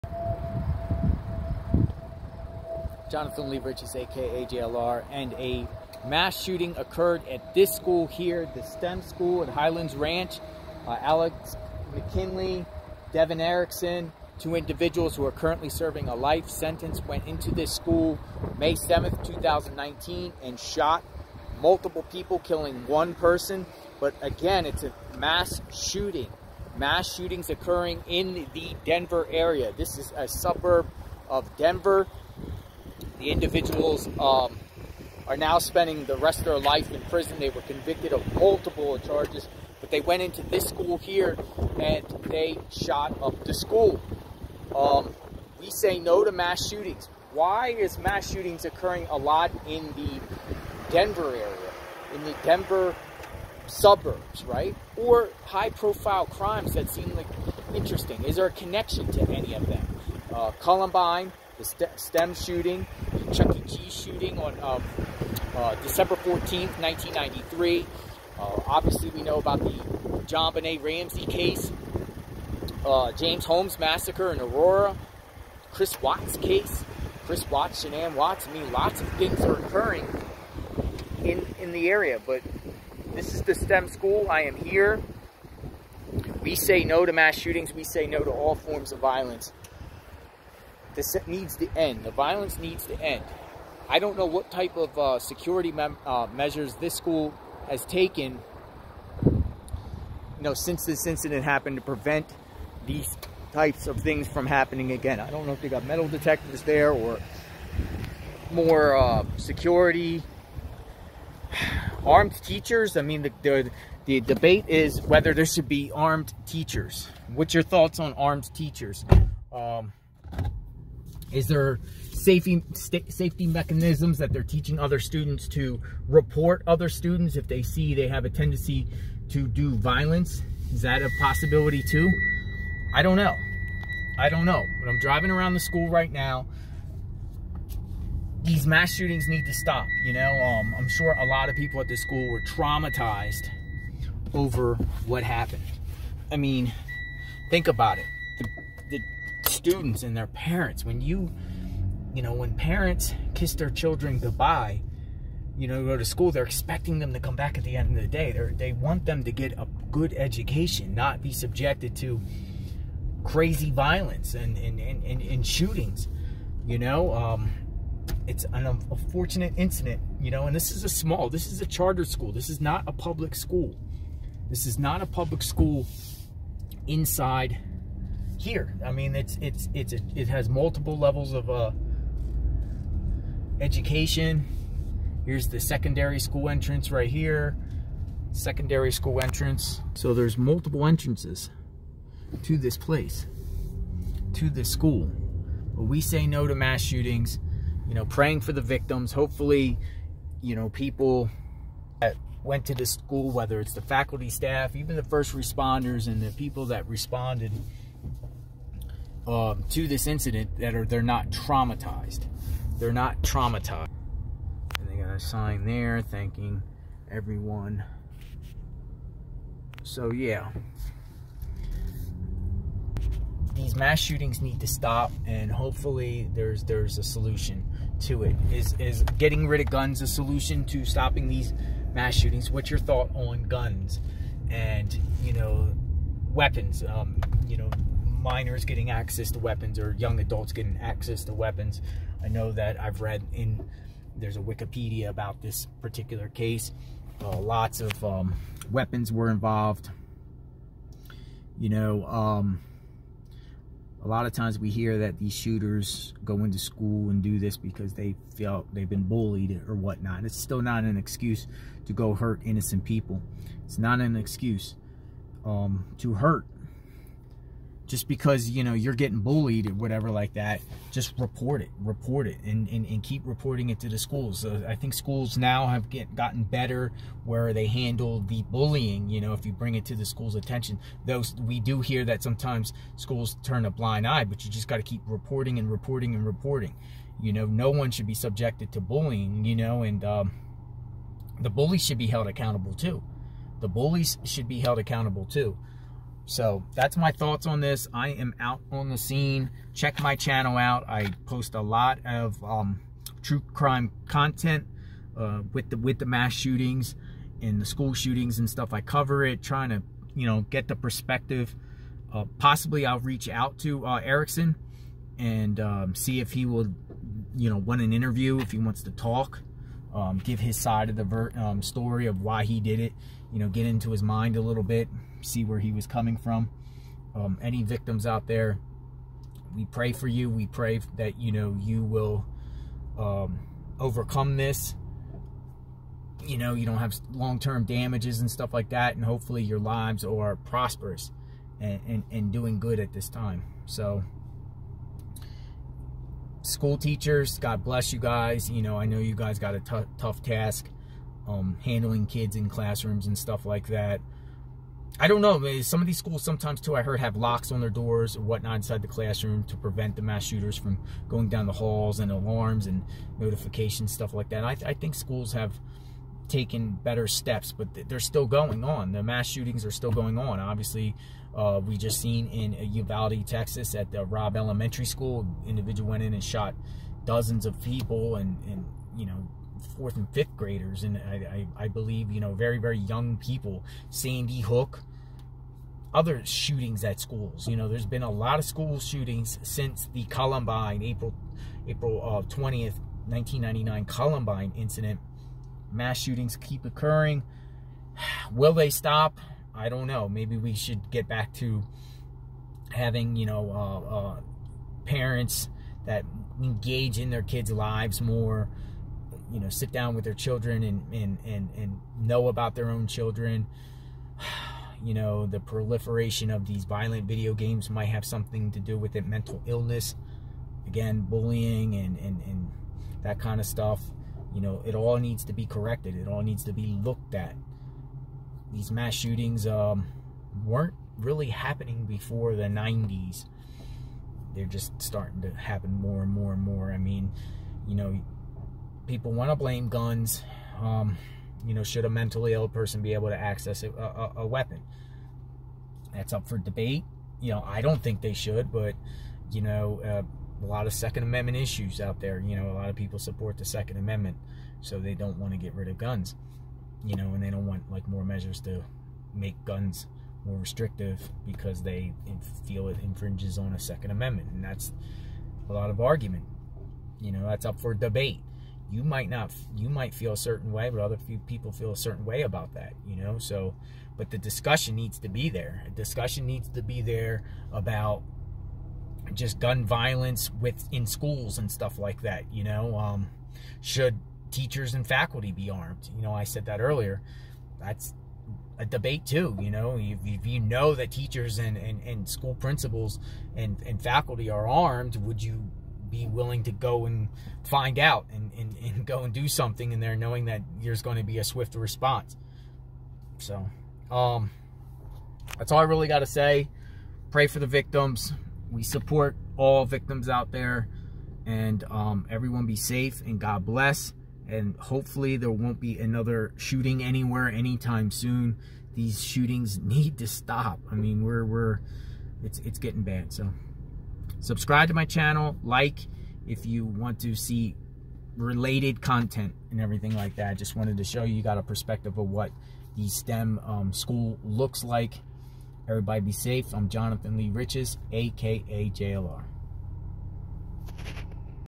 STEM School Highlands Ranch / Alec McKinney & Devon Erickson Discussion! Colorado
Praying for the Highlands Ranch / Denver Community!